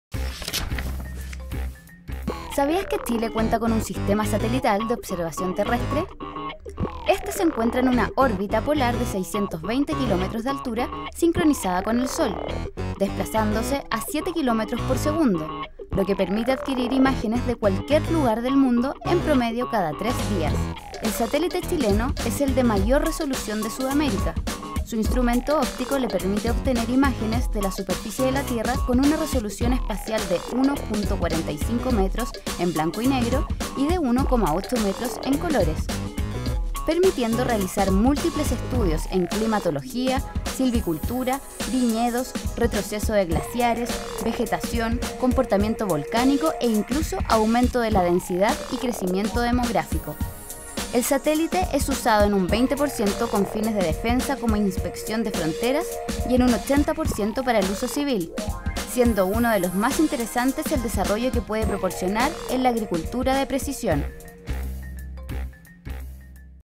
Un satélite chileno en el espacio
Fasat Charlie es el nombre del satélite chileno que recorre la órbita de la Tierra para múltiples funciones.